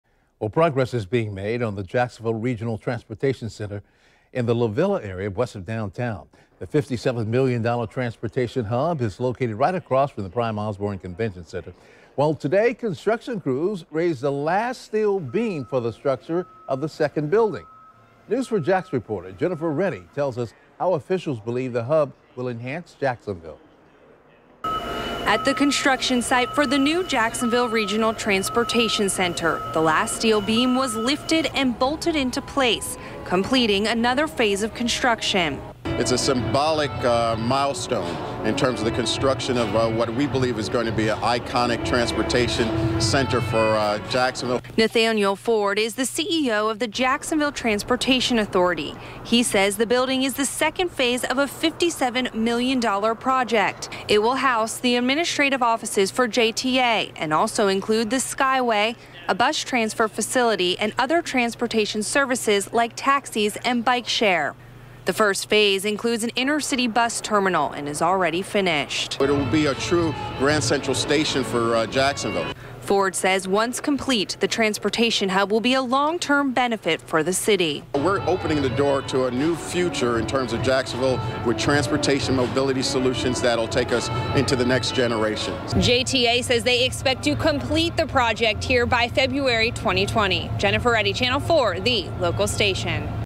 Jacksonville transportation hub project moving along
Progress is being made on the Jacksonville Regional Transportation Center in the LaVilla area, west of downtown.